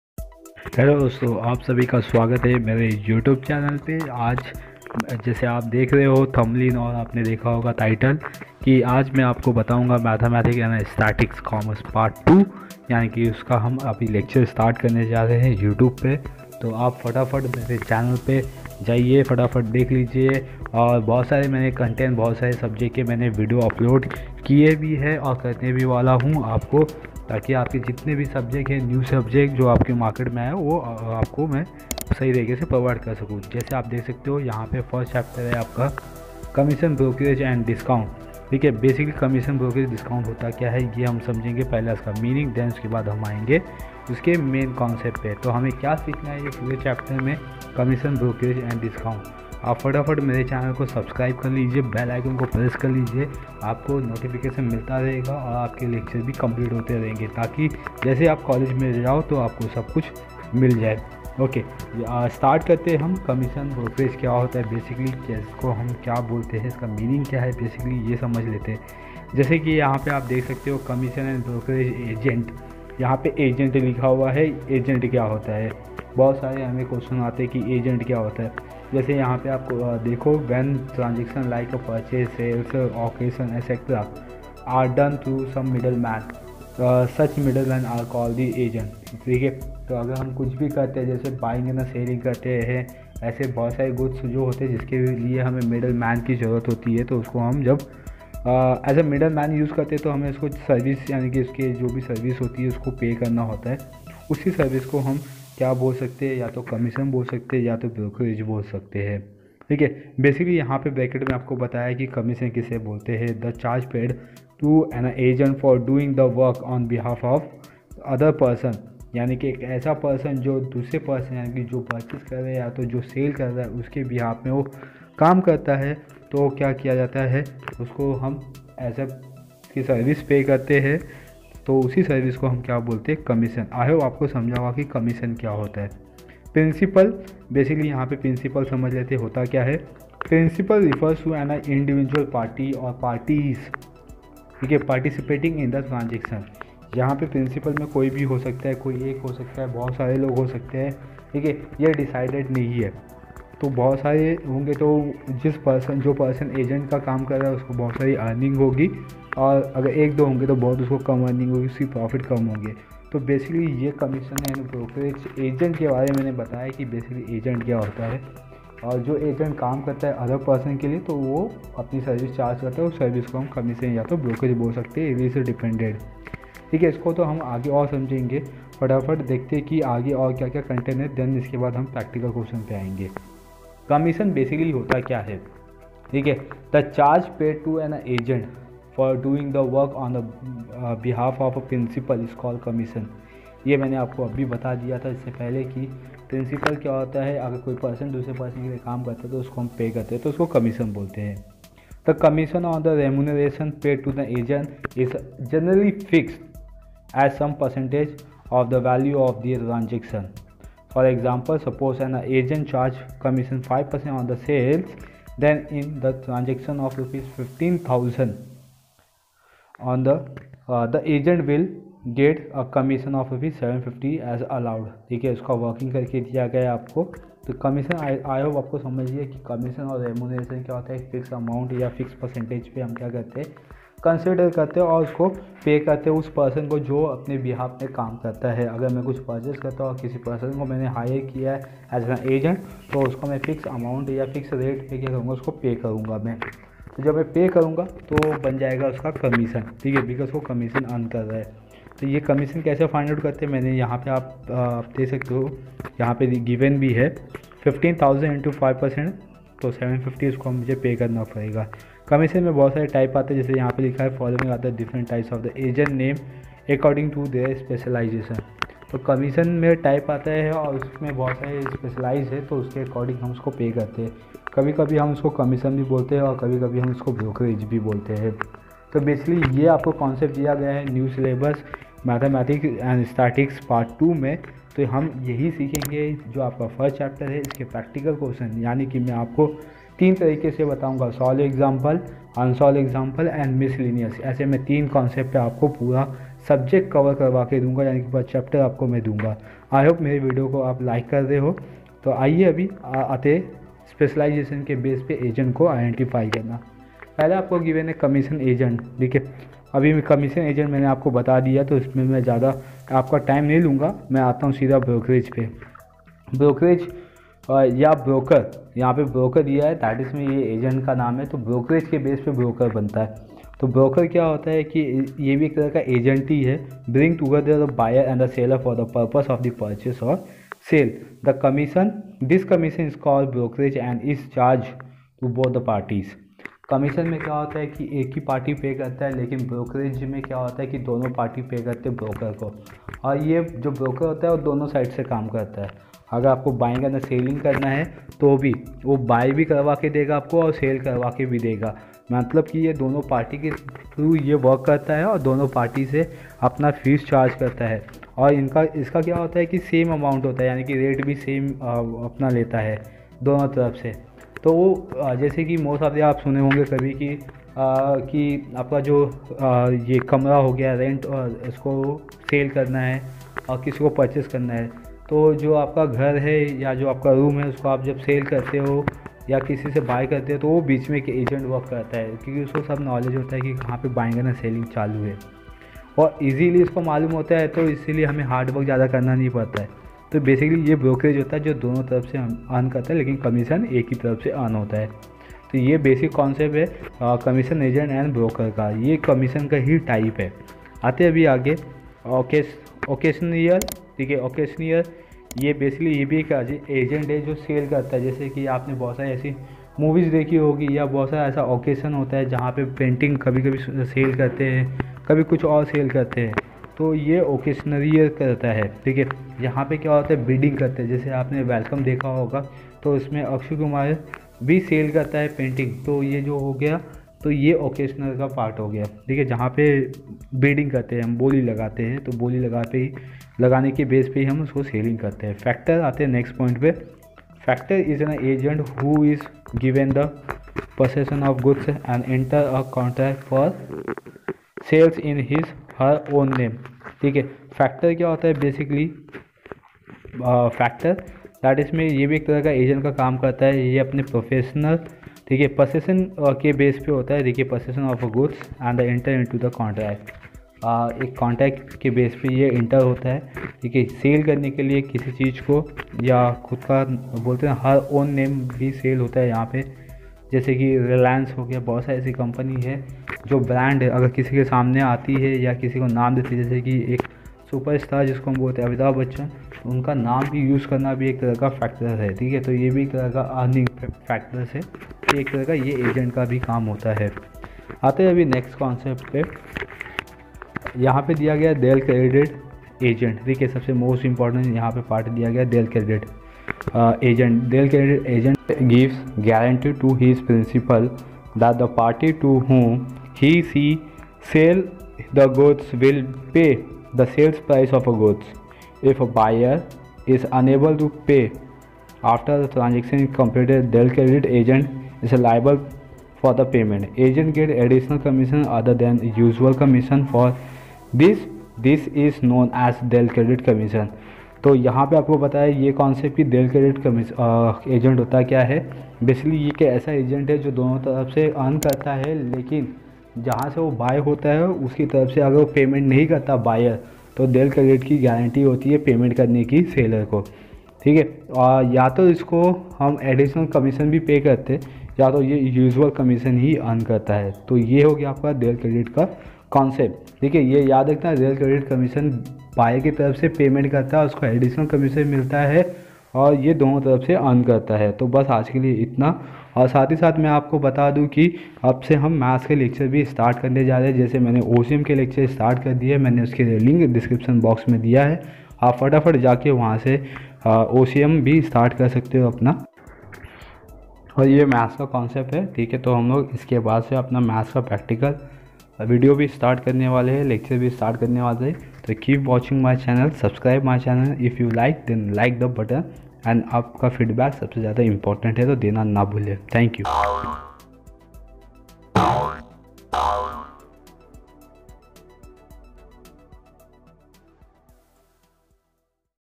Commission, Brokerage and Discount ll Mathematics and statistics ll HSC ll Part 1
12th STD Maharashtra Board new syllabus Commerce Mathematics and statistics (part -2)
It's important video. you will learn Chapter 1, Commission Brokerage and Discount (Practice set 1.1).

This is First video of Commission Brokerage and Discount. Remaining 2 video link given below 👇

All Questions will be Solve
All Questions will be Explain in detail

New Syllabus Review